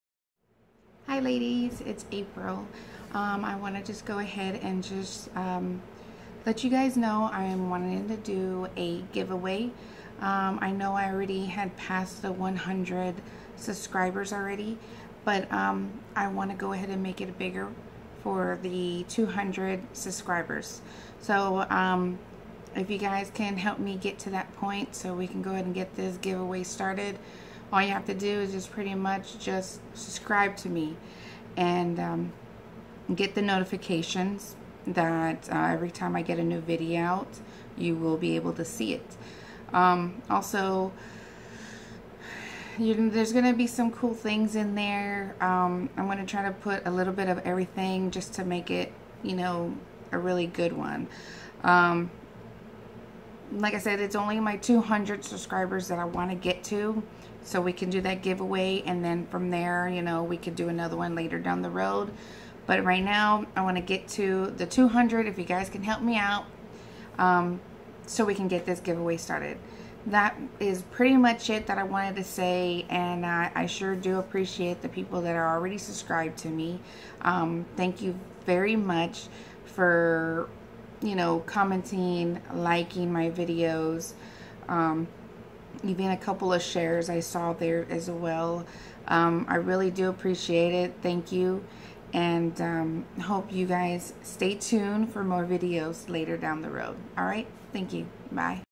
Help me to get this GIVEAWAY started
. Its gonna have polishes ,nail files, plates, stampers , scapers, top coats, base coats pretty much a little of everything.  So please help me get to that point so we can start having some fun.

Thank you all and have a great day. . Bye